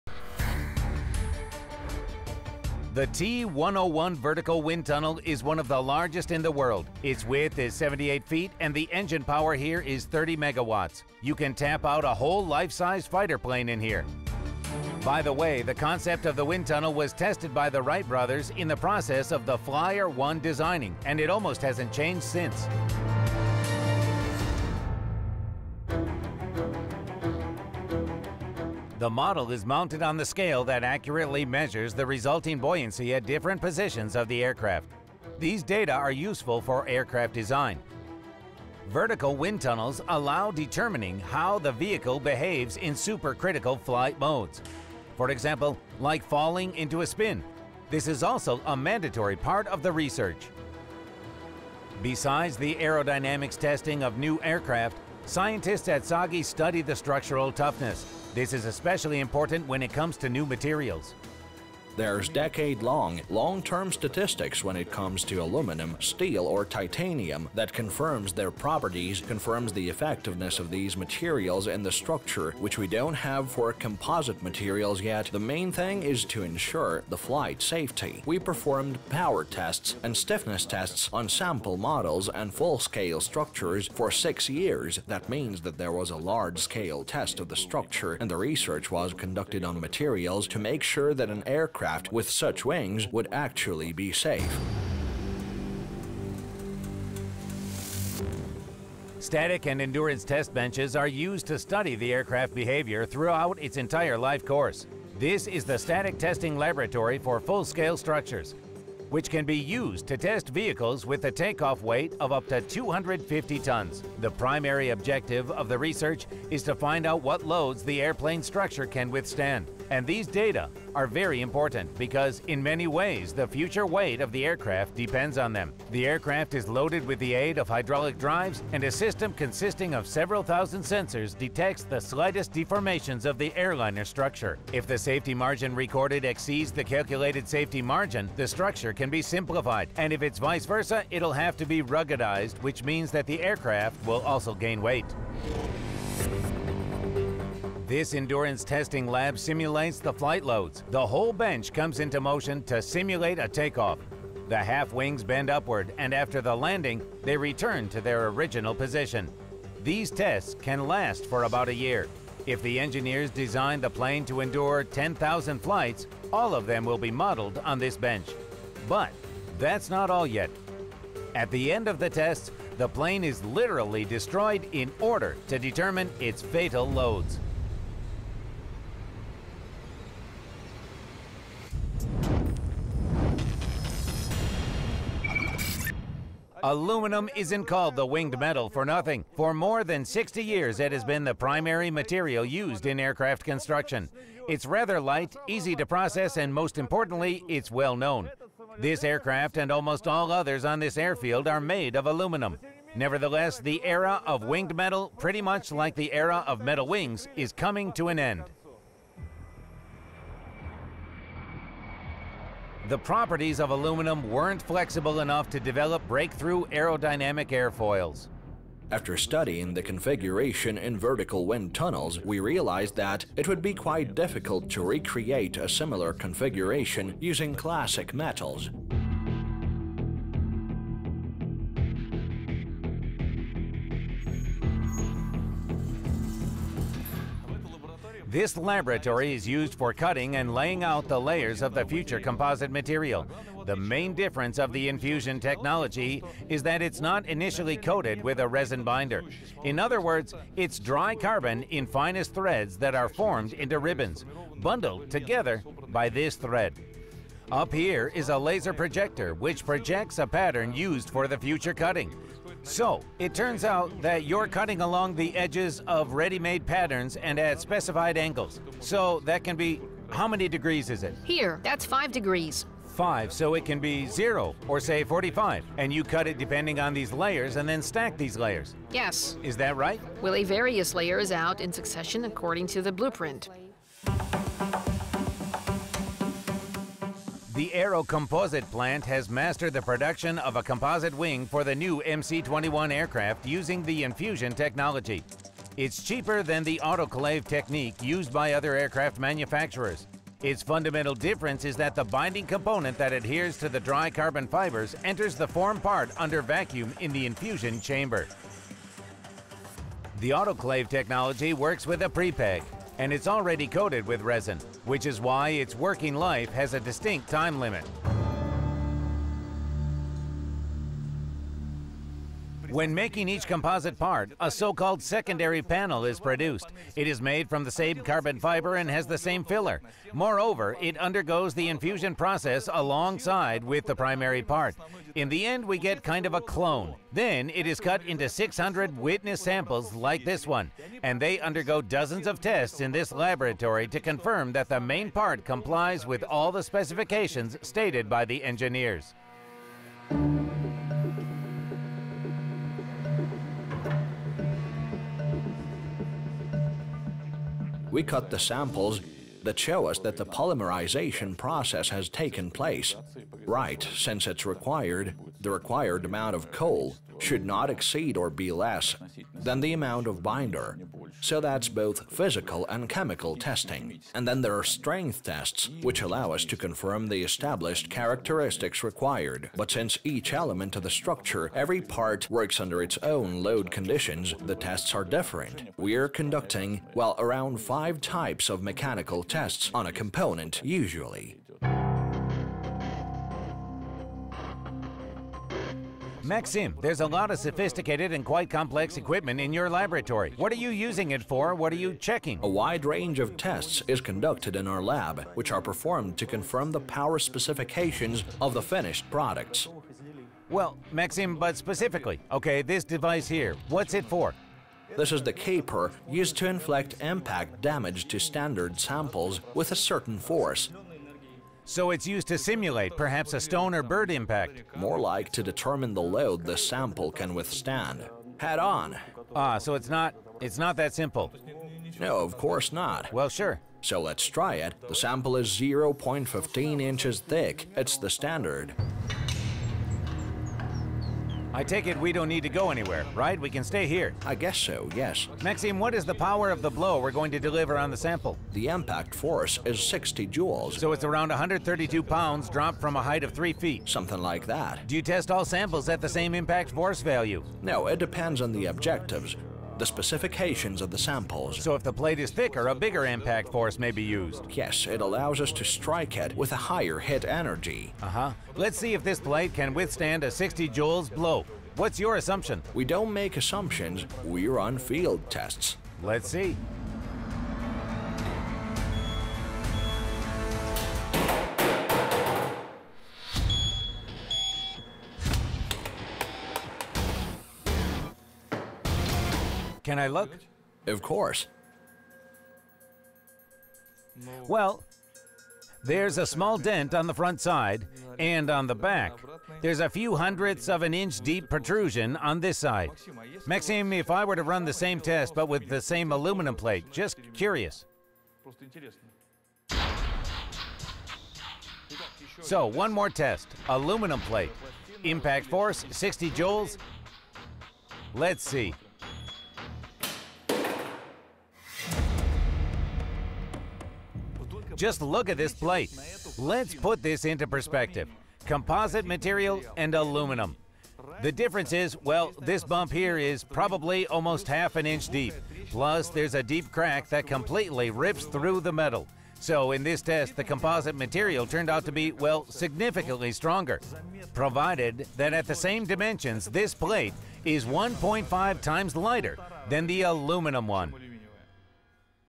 Inside One Of The LARGEST Aircraft Testing Facilities
One of the world’s biggest wind tunnel where airplanes are tested.